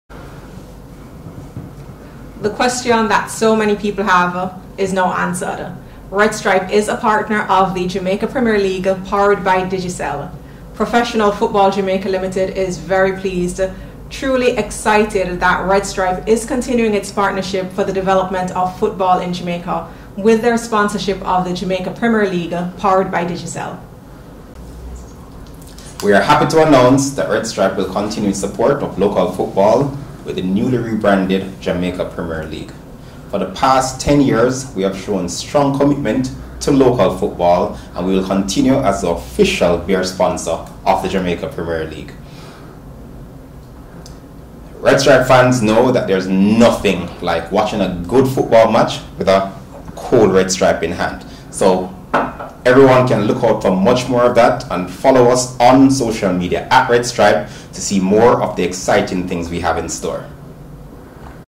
Red Stripe signs new deal with Jamaica Premier League
Long-time football sponsor Red Stripe has indicated its continued commitment to the sport with a new sponsorship agreement with the Jamaica Premier League, powered by Digicel (JPL). The announcement of the beer company’s support of the newly rebranded league came on Wednesday with an official inking of the deal at the Jamaica Pegasus. Red Stripe, a category sponsor, is thus the “official beer of the JPL”.